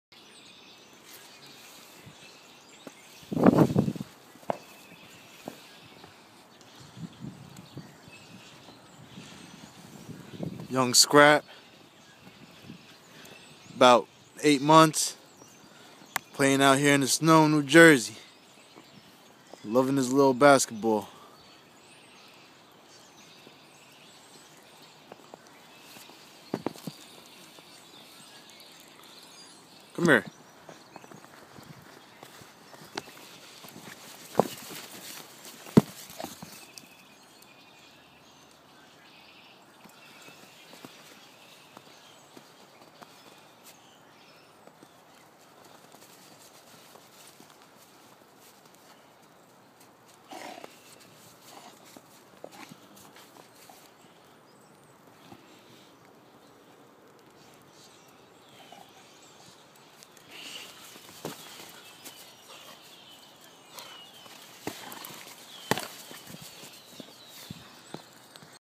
Young Scrap Rednose Pitbull
Scrappy playing in the snow in Neptune NJ on 3-8-13 he love dat flat basketball and snow lol he's about 8 months in this video.